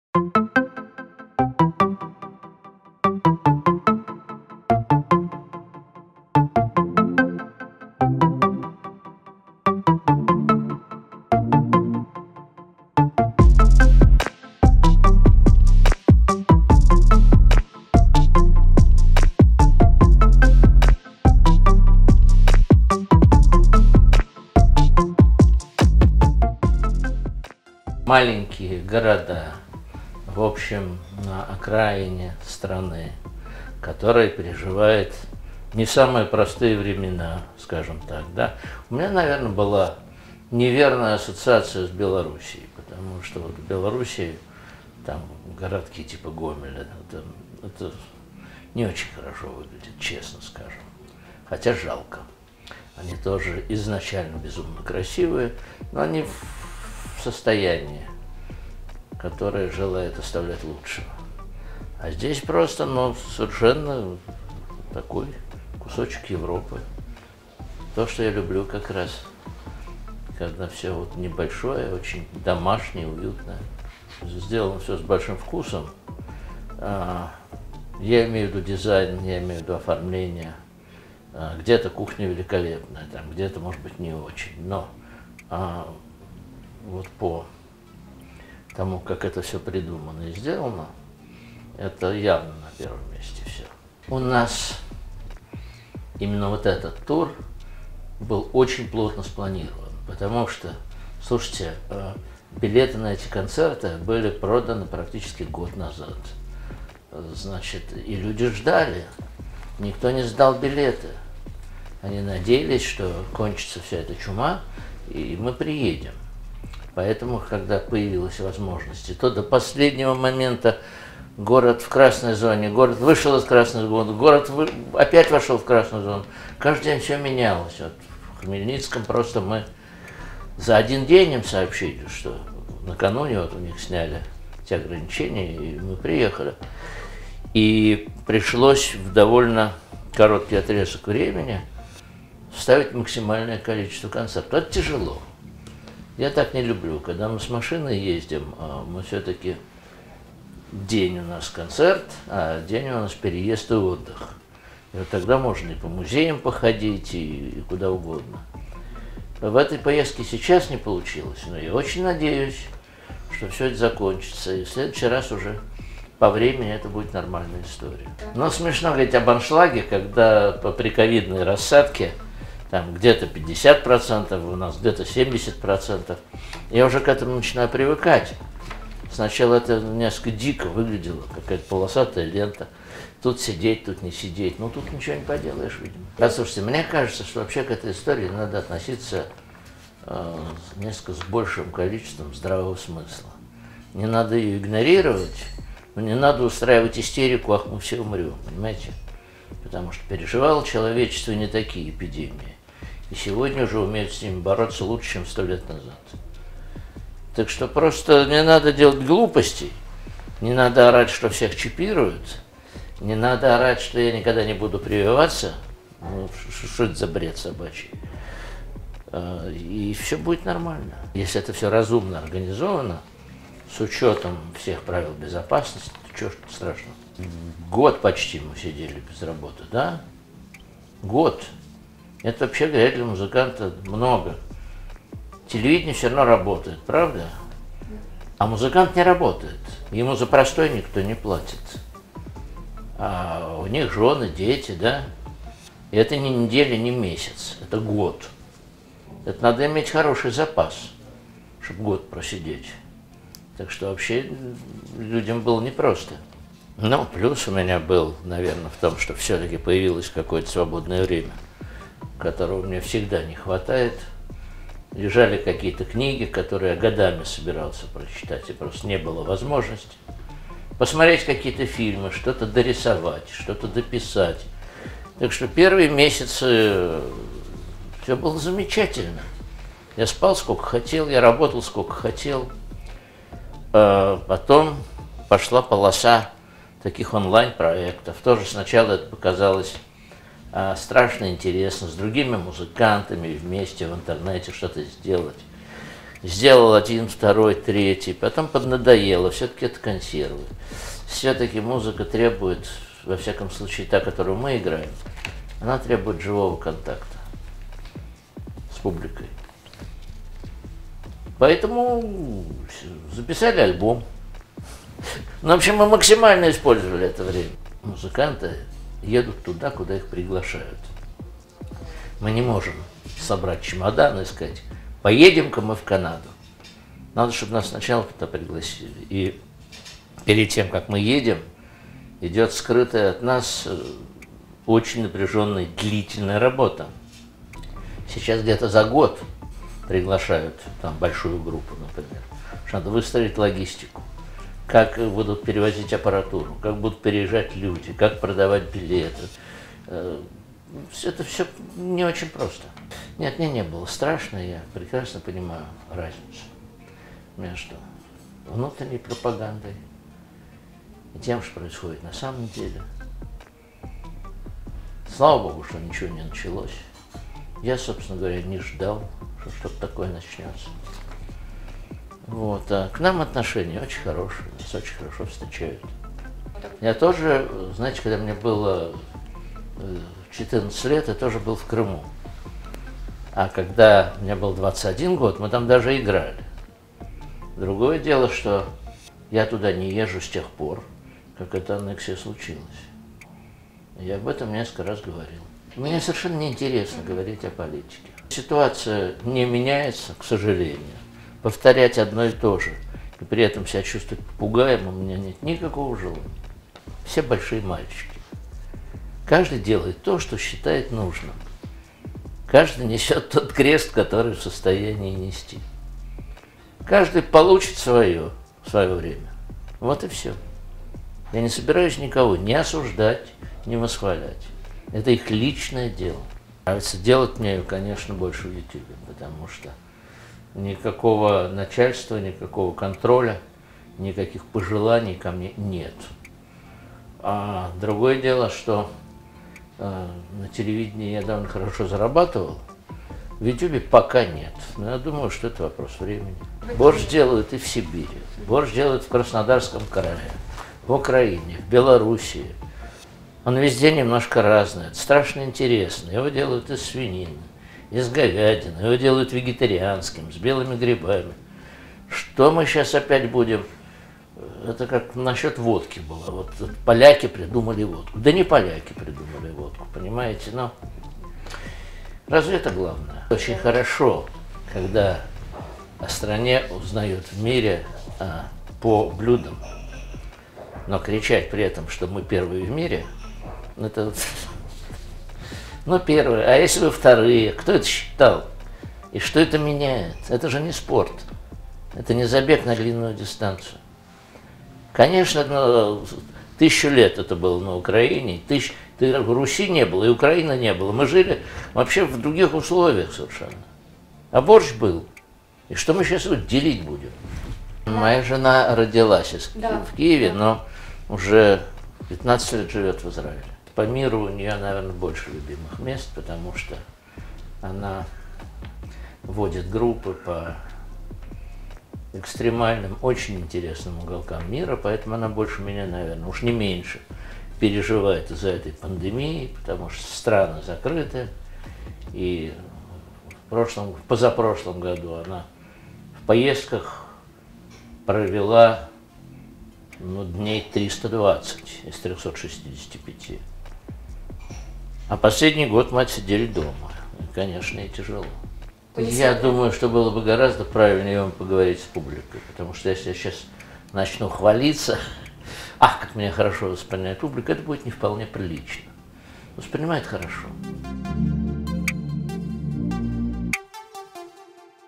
Мне не страшно ехать в Украину: Андрей Макаревич о туре, пропаганде и карантине
Российский музыкант и лидер группы Машина времени Андрей Макаревич рассказал о туре по Украине с коллективом Yo5 и о плюсах и минусах карантина.

Также артист поведал о своем кулинарном YouTube-проекте Смак и высказался о популяризации украинского борща.

Андрей Макаревич признался, что его последний тур по Украине очень плотно спланирован, поэтому погулять окрестностями городов у него времени нет.